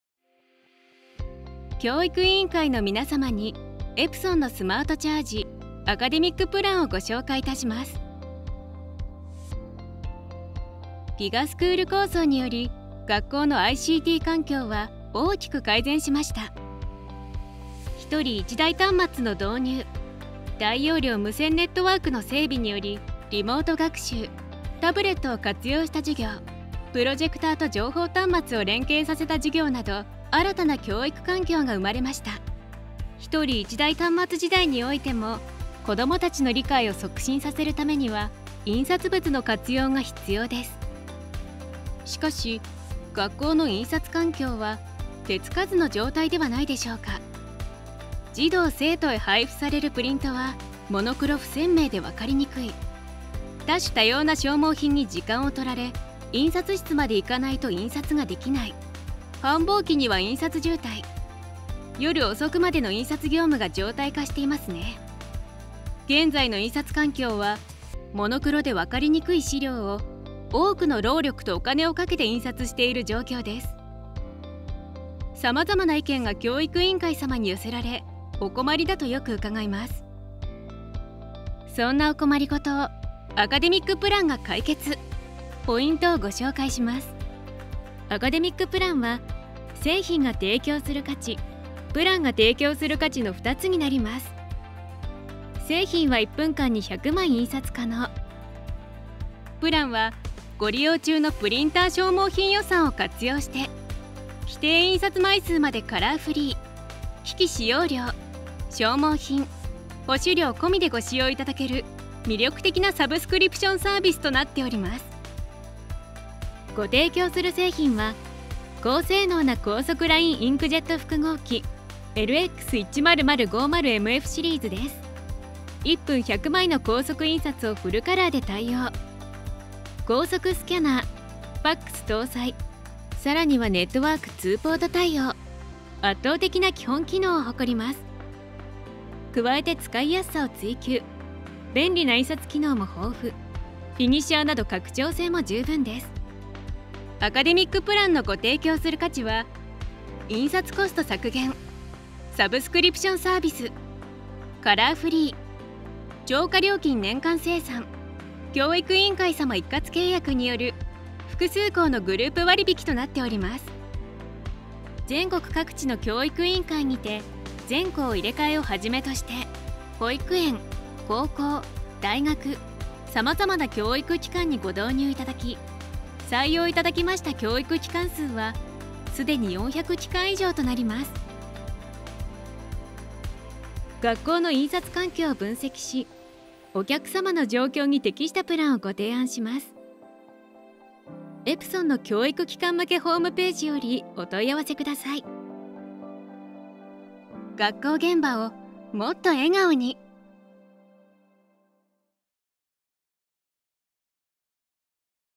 エプソンの学校向け『アカデミックプラン』のご紹介です。_1206705364220
エプソンの学校向け『アカデミックプラン』のご紹介です。『学校現場をもっと笑顔に』ご提案します。